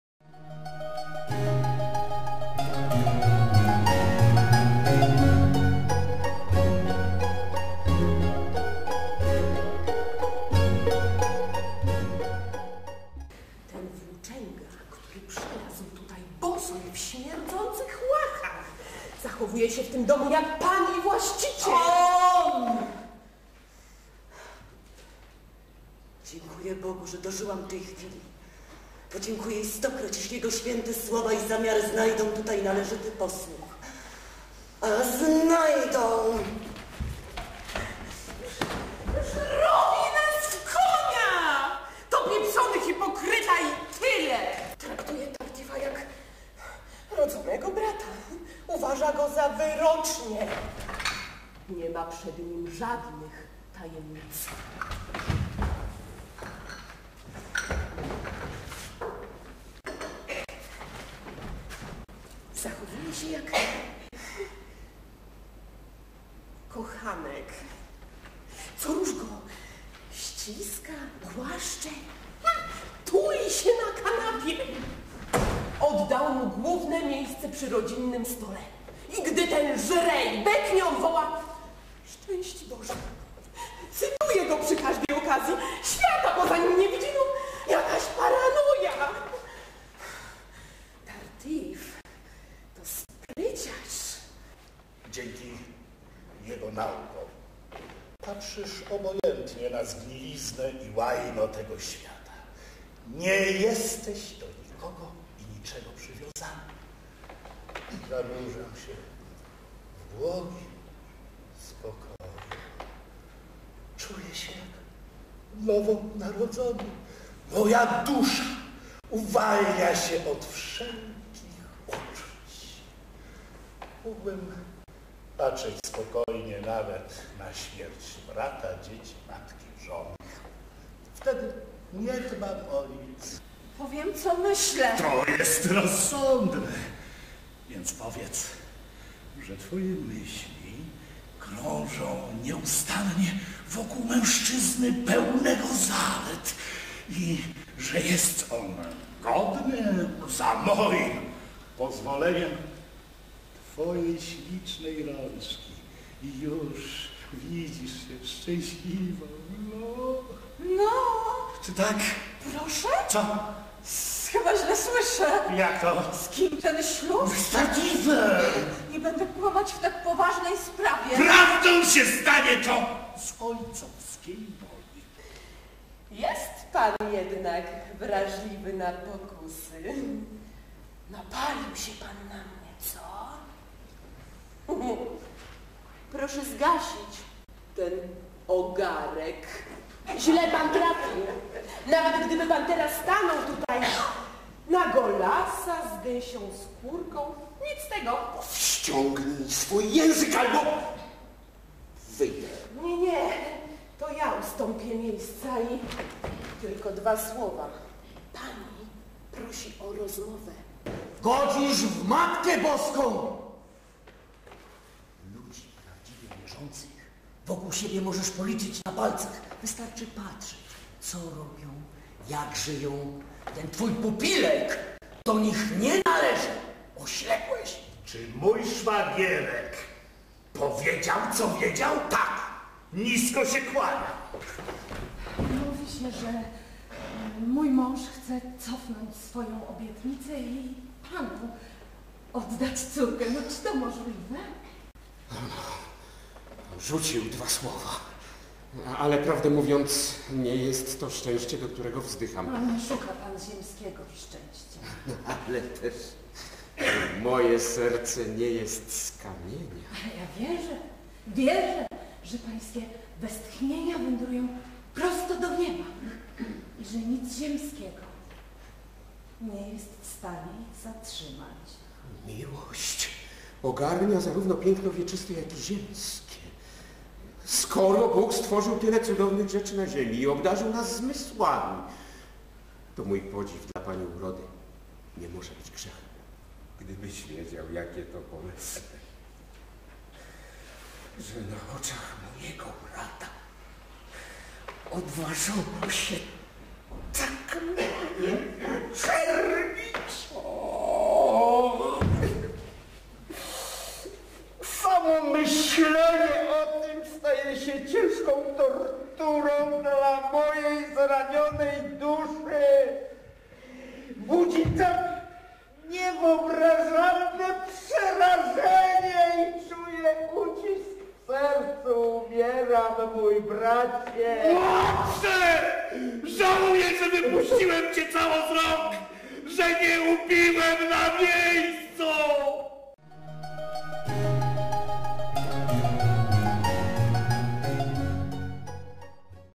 ŚWIĘTOSZEK
"Swiętoszek" Moliera w Teatrze im. Fredry w Gnieźnie, w reżyserii Józefa Jasielskiego.
Realizacja video Maurycy Kłopocki Dragon Film